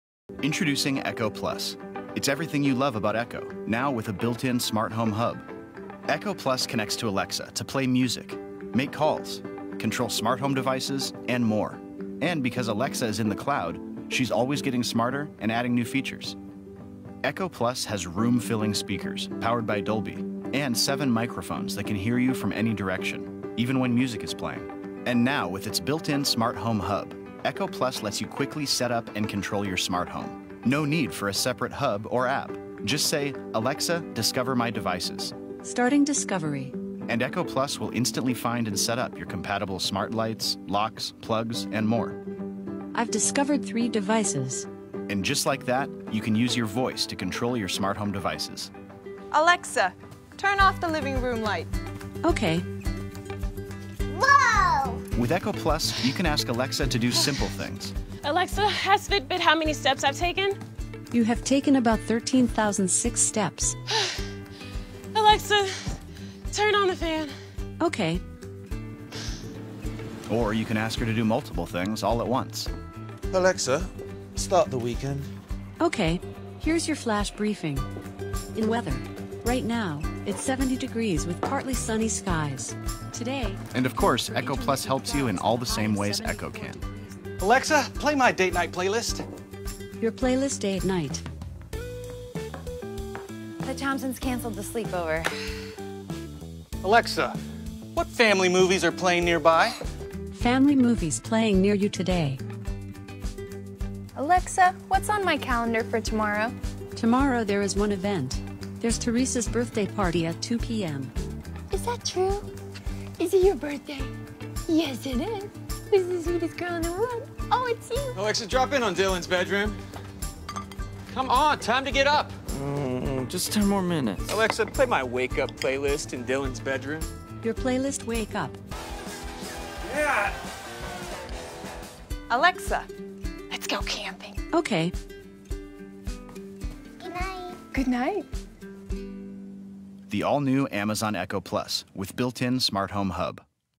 Echo Plus
Echo Plus connects to Alexa—a cloud-based voice service—to play music, make calls, set timers and music alarms, ask questions, check traffic and weather, and more—instantly.